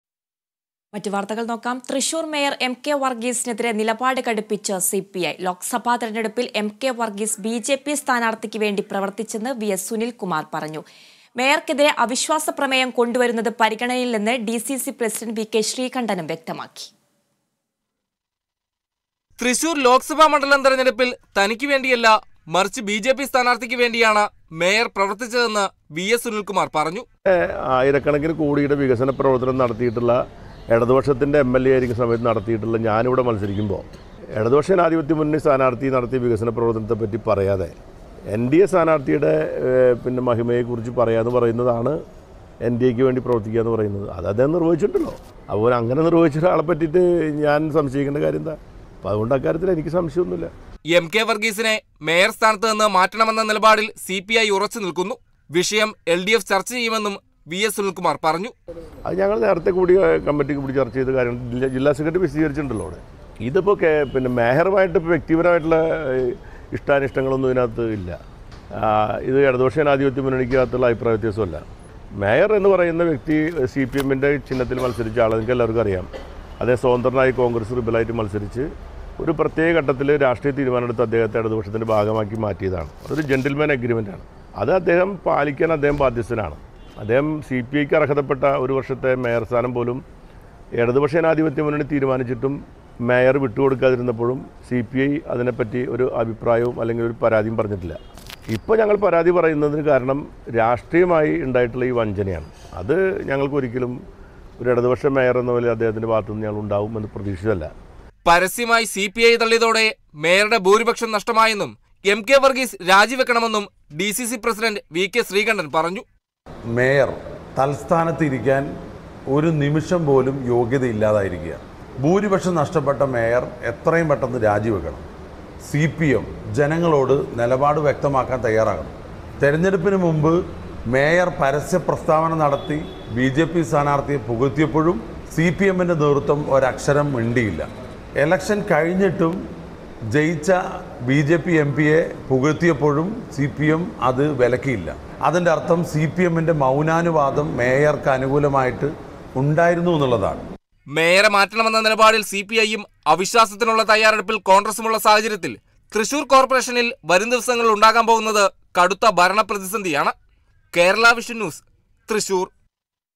തൃശ്ശൂര്‍ മേയര്‍ എം.കെ വര്‍ഗീസിനെതിരെ നിലപാട് കടുപ്പിച്ച് CPI
തൃശ്ശൂര്‍ മേയര്‍ എം.കെ വര്‍ഗീസിനെതിരെ നിലപാട് കടുപ്പിച്ച് സിപിഐ. ലോക്‌സഭാ തെരഞ്ഞെടുപ്പില്‍ എം.കെ വര്‍ഗീസ് ബിജെപി സ്ഥാനാര്‍ഥിക്ക് വേണ്ടി പ്രവര്‍ത്തിച്ചെന്ന് വി.എസ് സുനില്‍കുമാര്‍ പറഞ്ഞു. മേയര്‍ക്കെതിരെ അവിശ്വാസ പ്രമേയം കൊണ്ടുവരുന്നത് പരിഗണനയിലെന്ന് ഡിസിസി പ്രസിഡന്റ് വി കെ ശ്രീകണ്ഠനും വ്യക്തമാക്കി.

About COA